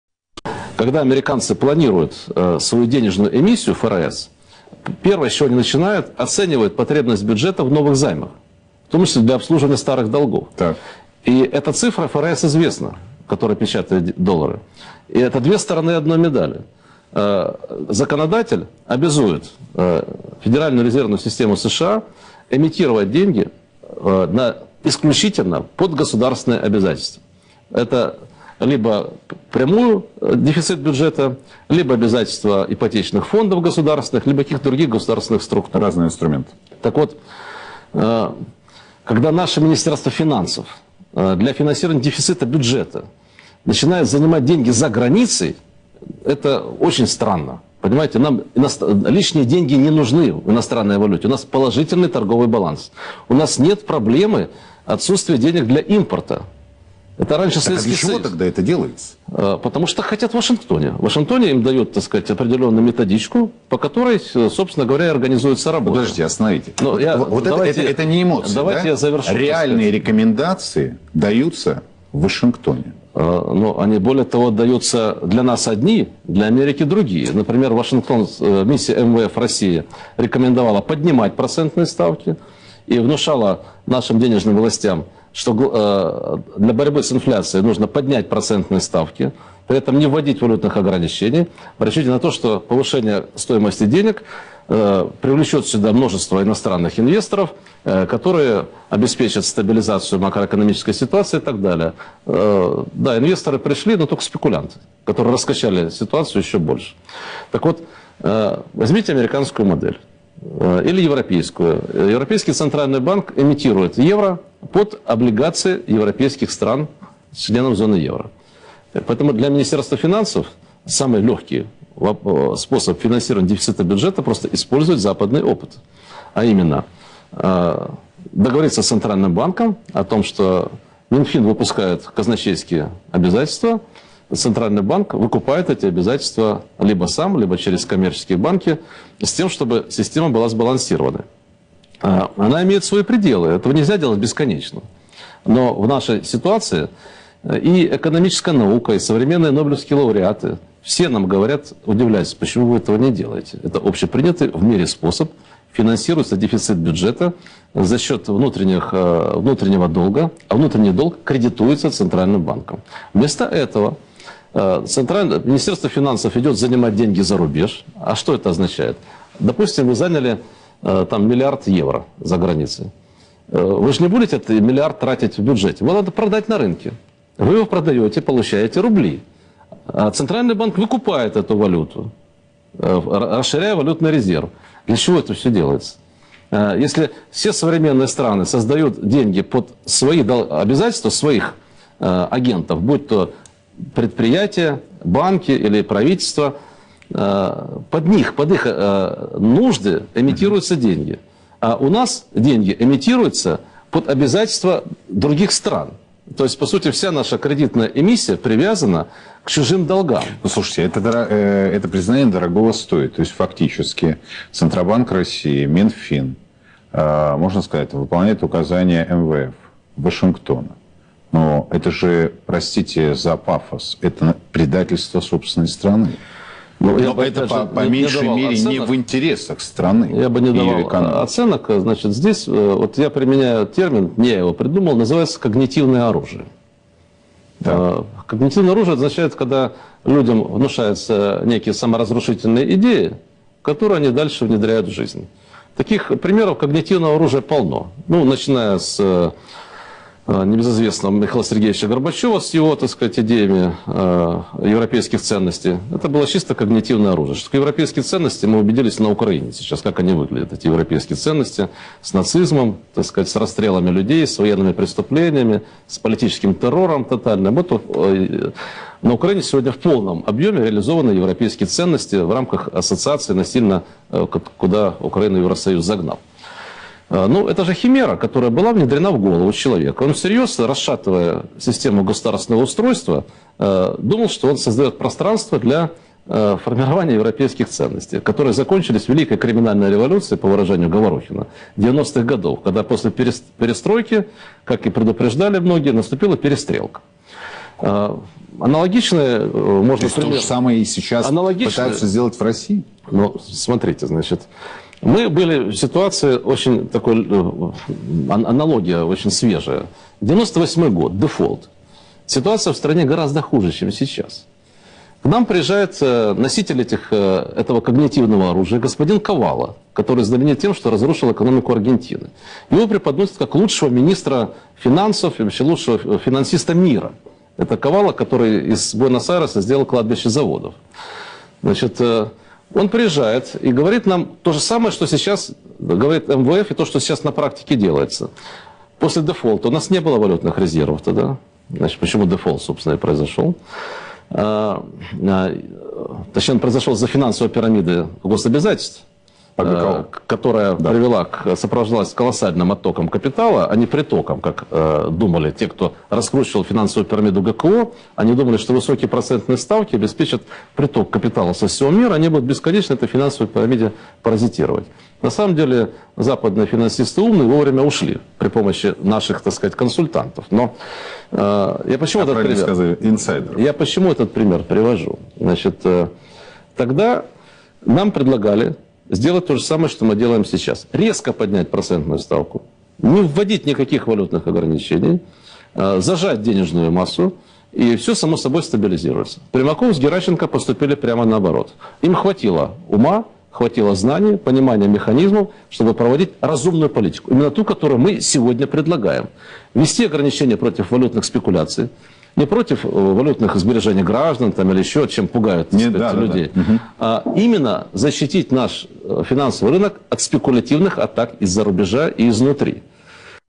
Вышка 4•Факультет свободных искусств и наук•Реальное время
Ценные указания (ЦУ) из-за бугра. Почему для России ЦУ одни. Для самих авторов ЦУ - ЦУ совсем иные. Всё наоборот… Кто правит балом. Кто послушно исполняет. Почему в России самое большое количество ошибок. Ошибок именно в сфере экономики. В России – 170 %, в США только примерно 12%. Трансляторы ложных идей - кто такие. Кому выгодно. Смотри и слушай людей, болеющих за Державу. Видео ниже.

Это надо знать и помнить. «Когнитивное оружие» - что это такое. Читай: - Александр Жилин, Национальные угрозы России,

Всё и даже больше найдёшь...
Читай пресс-релизы. Здесь прямые  ссылки::
Удачи
Юлия Кантер